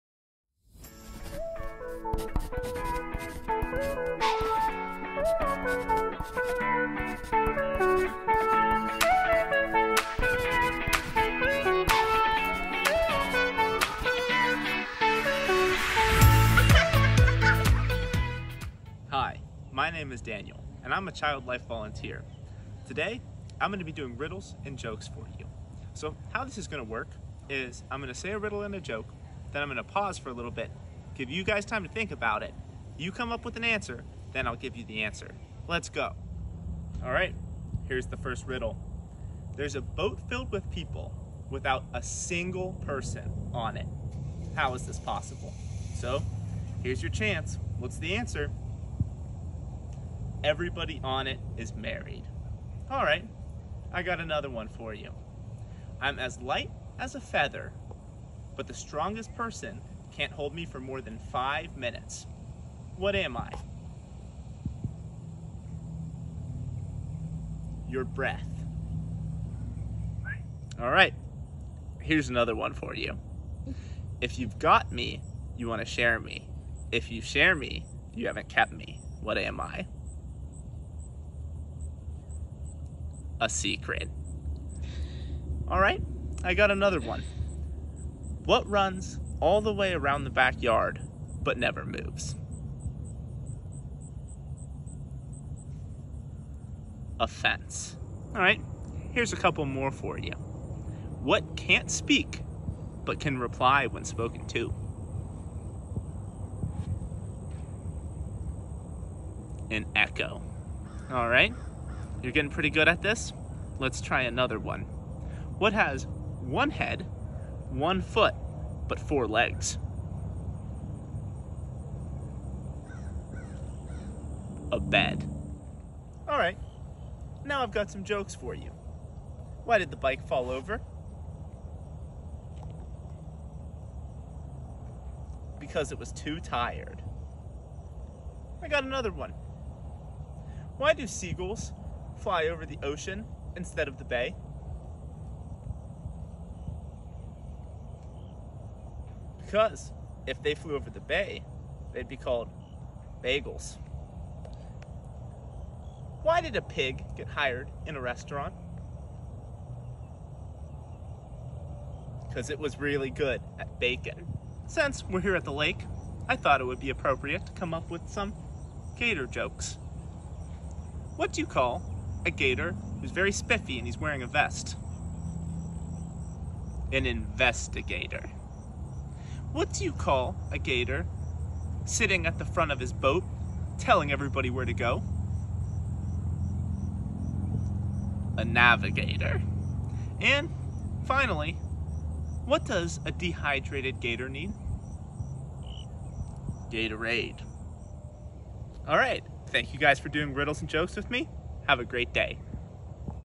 Fun Riddles and Jokes!
In this video, UF Health Shands Children's Hospital Child Life Volunteer, Daniel, will ask you some silly riddles, and tell you some funny jokes! Follow along as he asks tricky questions, and guess what the correct answers are! Feel free to pause the video at any time to think of some clever answers!

Video Edited by UF Health Shands Children's Hospital Child Life Volunteer, Kyara.